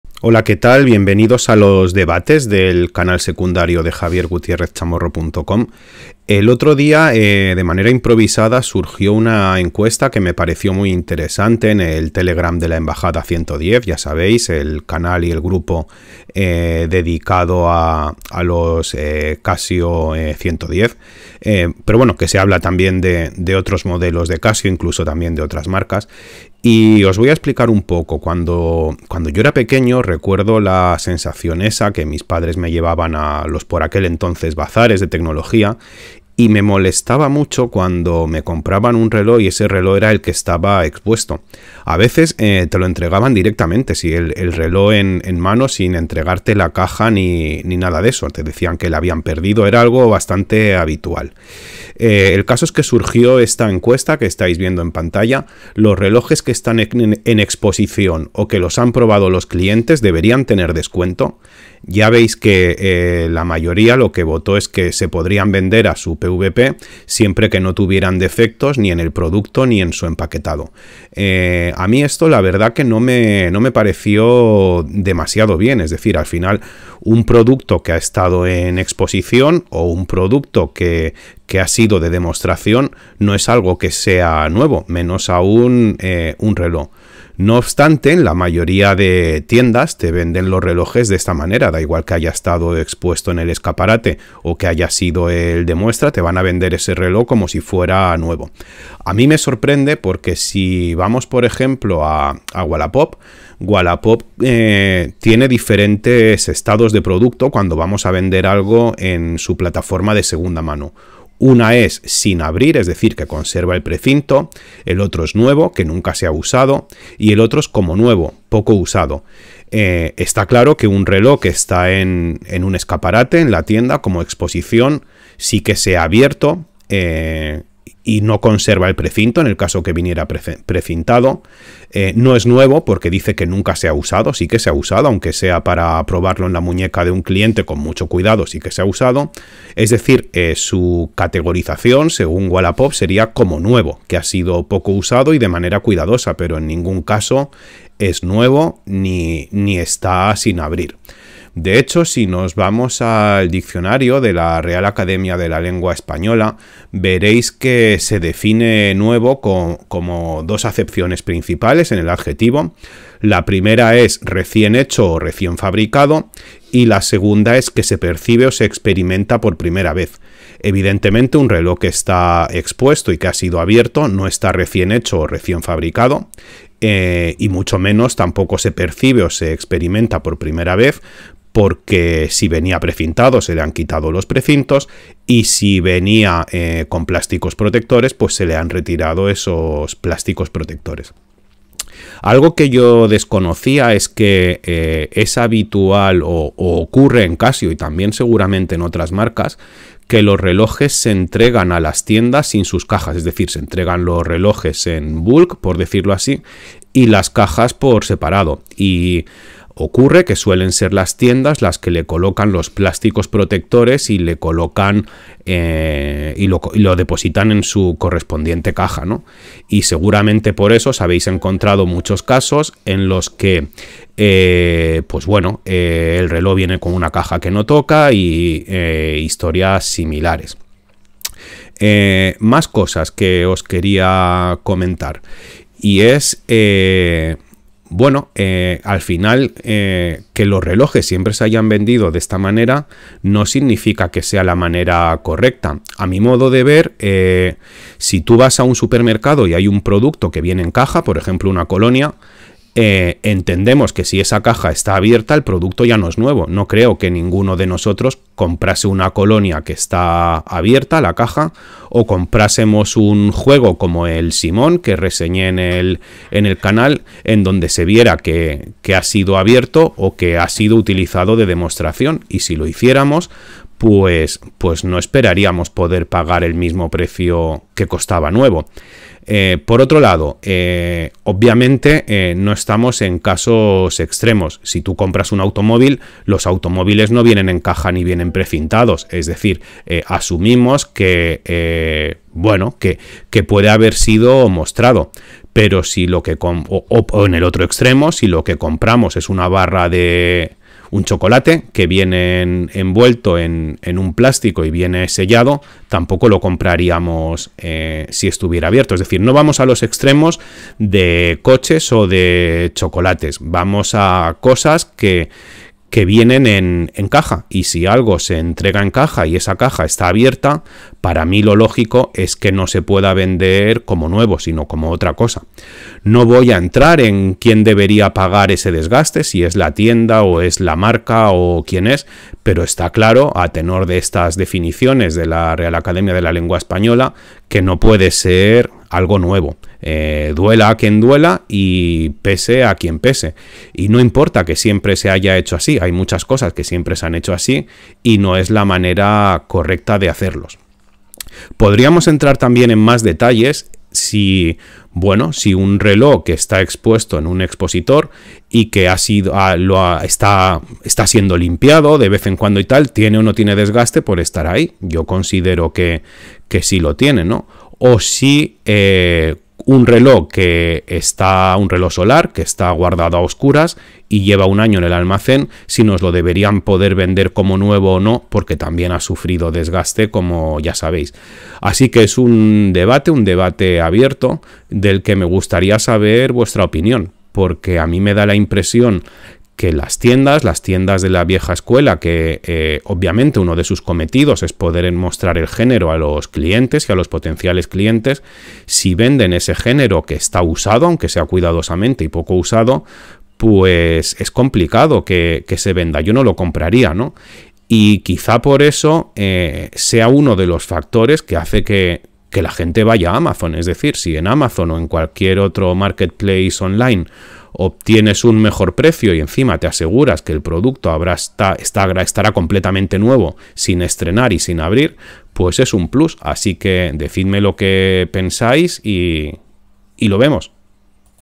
Un reloj que está en exposición, ¿se puede considerar nuevo?
En el grupo de Telegram de la Embajada110 surgió un interesante debate. ¿Un reloj que está en exposición es nuevo?

Aquí os explicaré mis argumentos... Dejadme vuestra opinión.